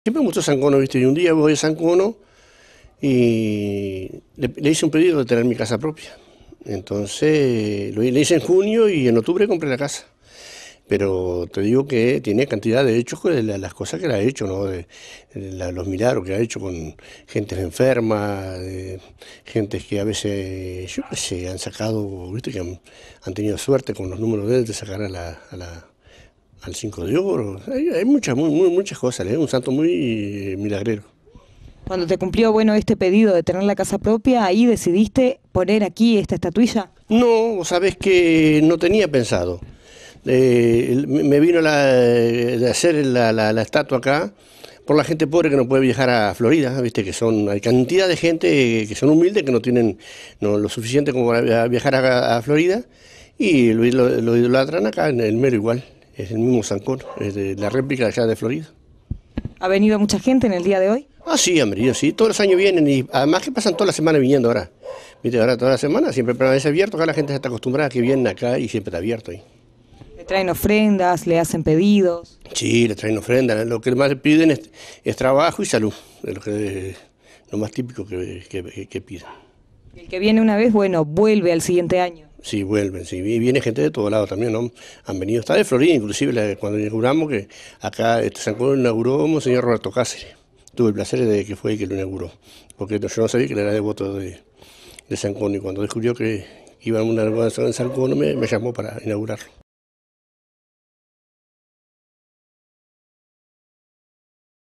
DIA DE SAN CONO
Como cada 3 de Junio  San Cono convoca a sus devotos. Quienes no viajaron  a Florida tienen en Melo la oportunidad de visitar la replica de su imagen, la que se encuentra en Barrio Arpí.
Enrique Charquero,  propietario de la finca donde se encuentra la  imagen de San cono hablo con nuestro informativo sobre la importancia  de  este santo en la vida de sus fieles.